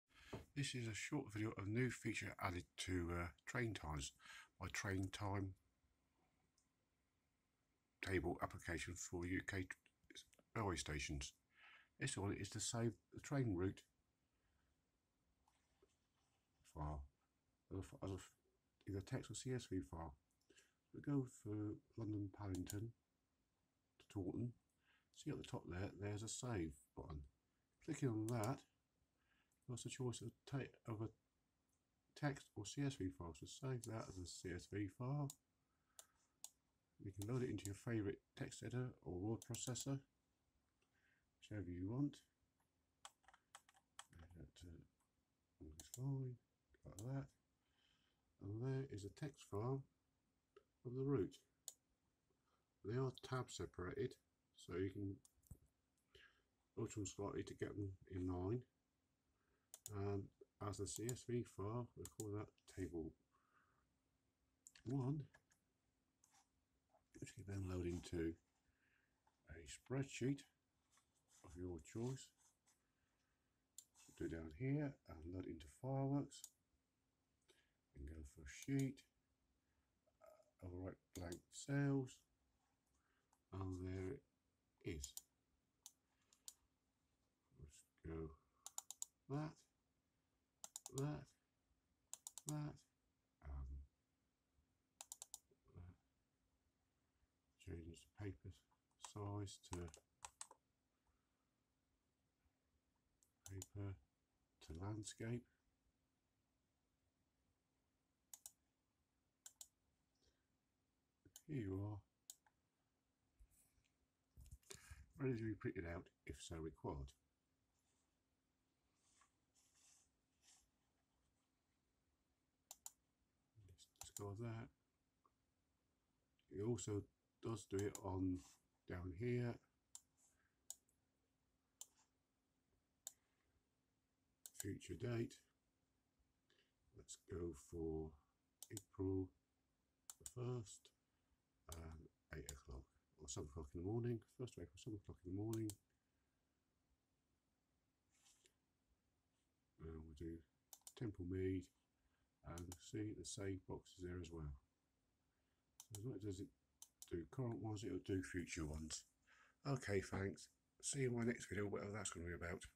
Save Route Added To TrainTimes
This is a short video of the latest update to my UK train time application TrainTimes bringing it up to version 1.08
The new feature is the ability to save the train route timetable in either plain text format or CSV format.